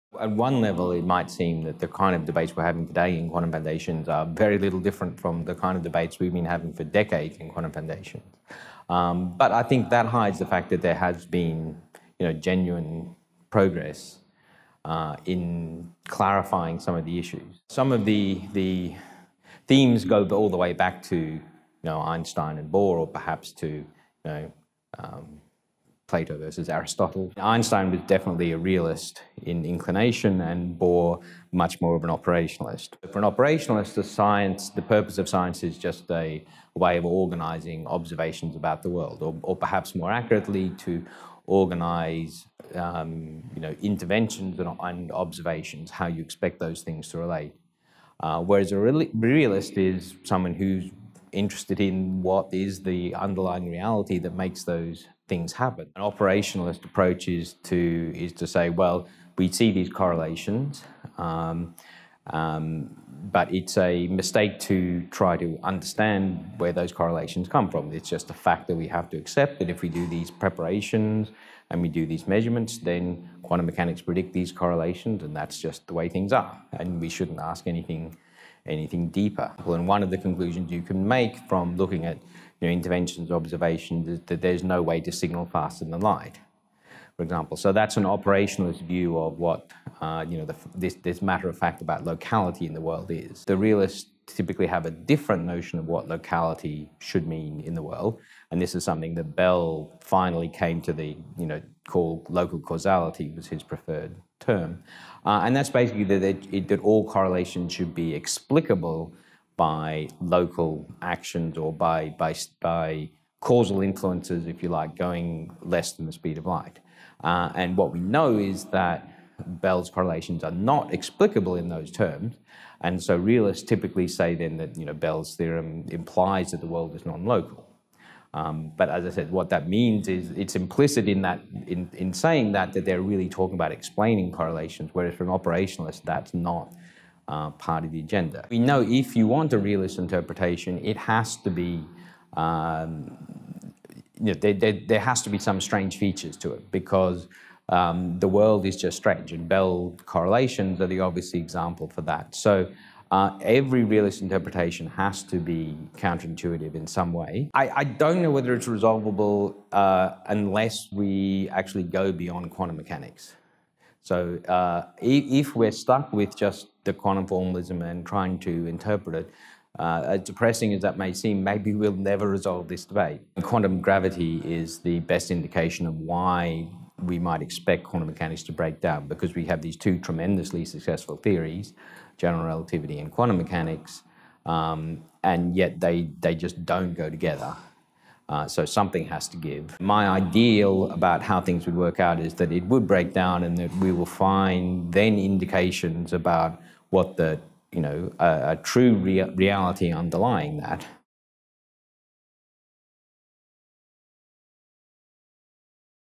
Interview with Howard Wiseman
Is nonlocality a deep feature of the quantum world, or just a misinterpretation? The answer awaits a unified theory of physics, which will go beyond quantum theory and show us what it really means, suggests theoretical physicist Howard Wiseman of Griffith University in Australia.